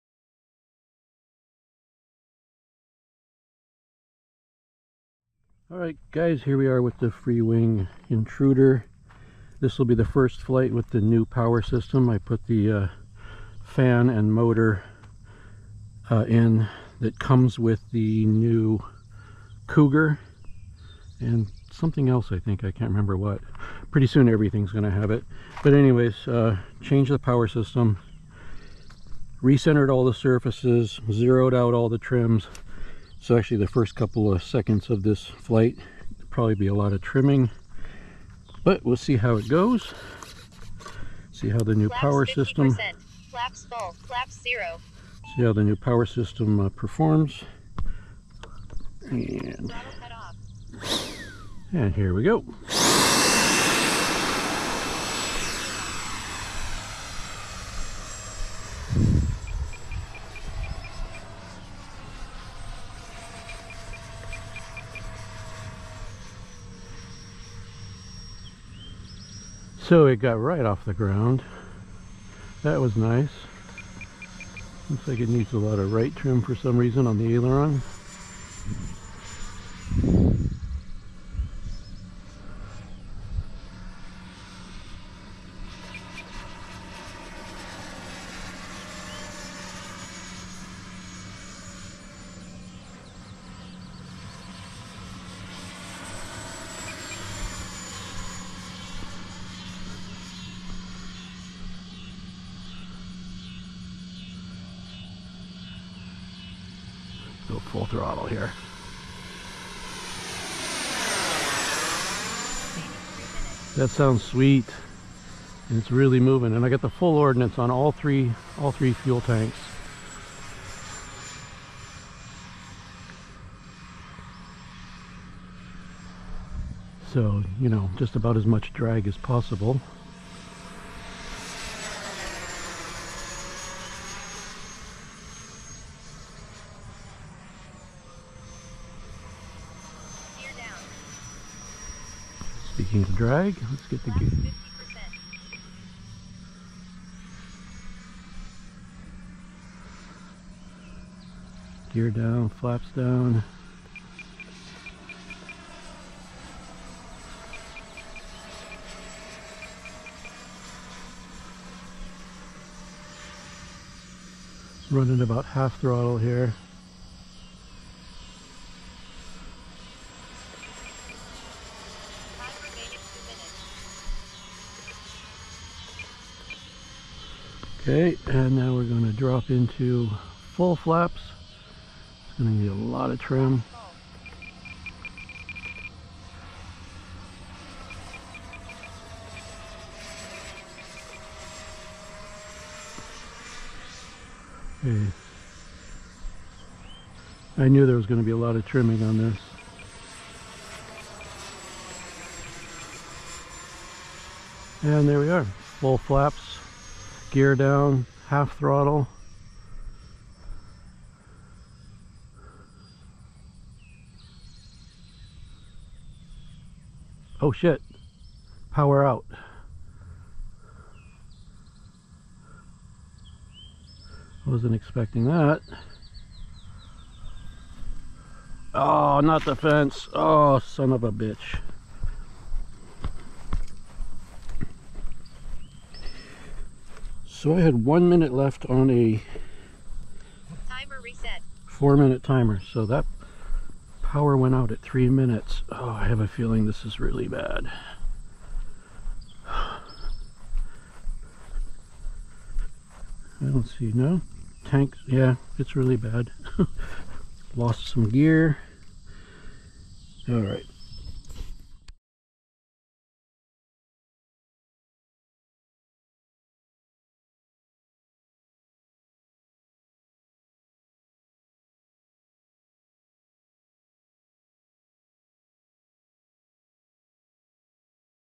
Freewing Intruder with AvantiS v2 Fan and Motor
A mistake was made ... but I got pretty lucky, it could have ended much worse.

Spoiler alert, the new power system worked great.